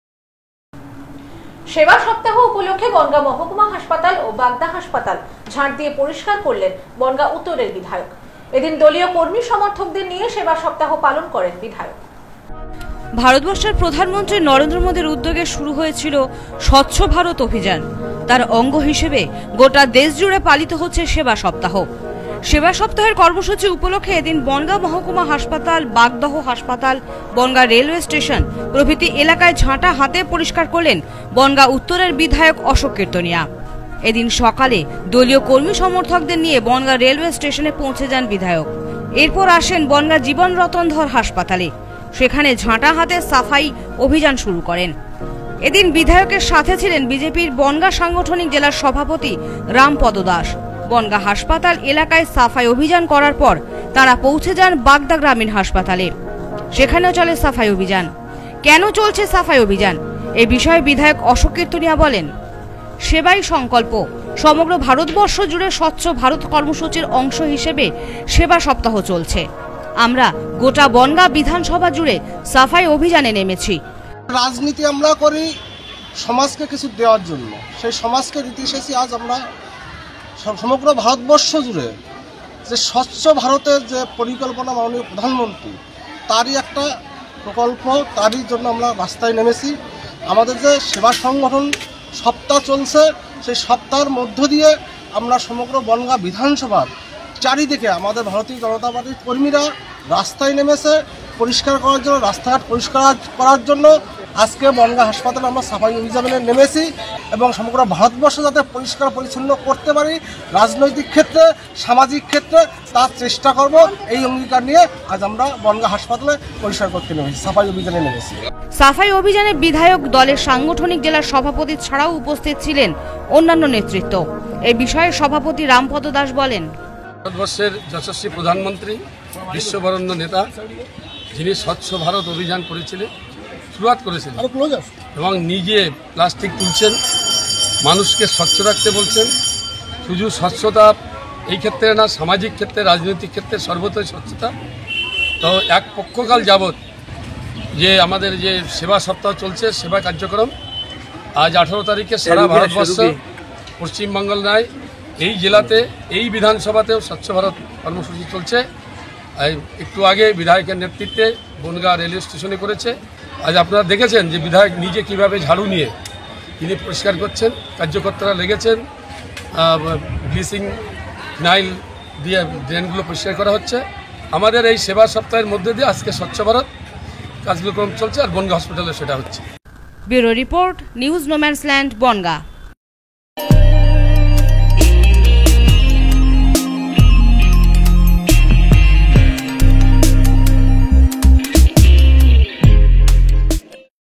বনগাঁ ও বাগদা হাসপাতালে ঝাঁট দিয়ে পরিষ্কার করলেন বনগাঁ উত্তরের বিধায়ক।
সেবা সপ্তাহ উপলক্ষে বনগাঁ মহাকুমা হাসপাতাল ও বাগদা হাসপাতালে ঝাঁট দিয়ে পরিষ্কার করলেন বনগাঁ উত্তরের বিধায়ক। এদিন দলীয় কর্মীদের নিয়ে হাসপাতালে গিয়ে পরিষ্কারের কাজ করেন তারা।